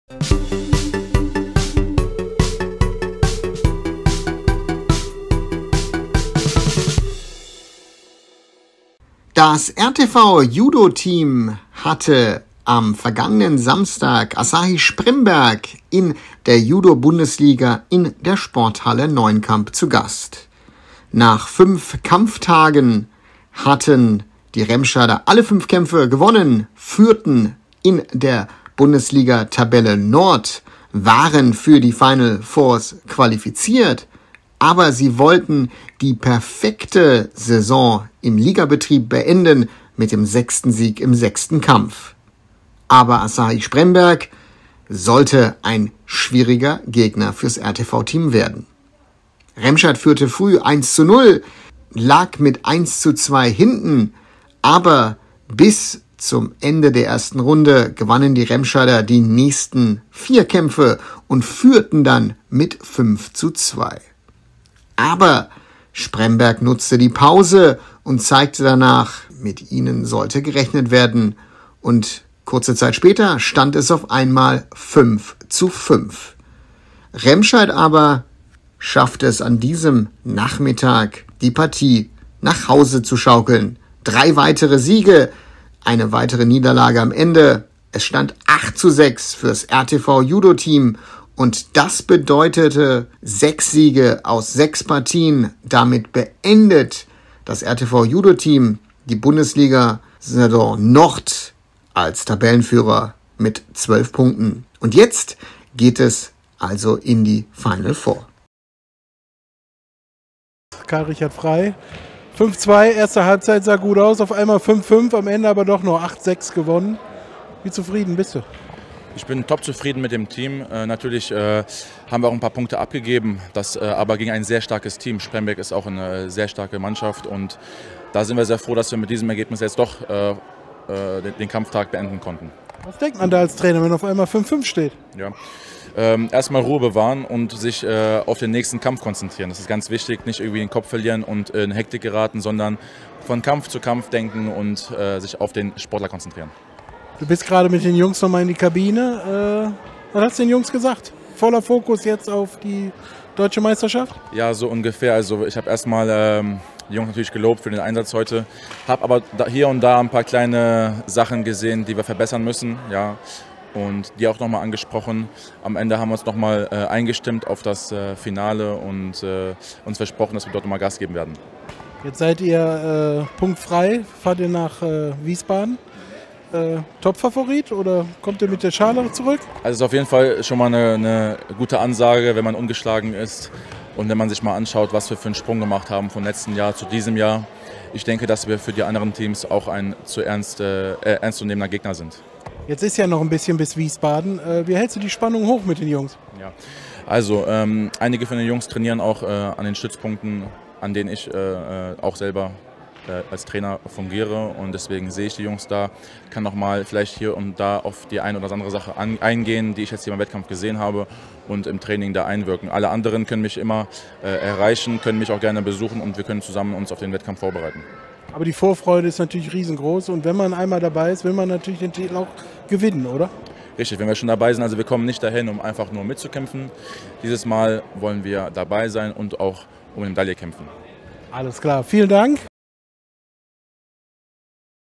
RTV JUDOTEAM besiegt Asahi Spremberg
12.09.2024 Das Remscheider TV JUDOTEAM hatte am vergangenen Samstag, Asahi Spremberg, in der JUDO Bundesliga in der Sporthalle Neuenkamp zu Gast.
Das RTV JUDOTEAM gewann am Ende mit 8-6.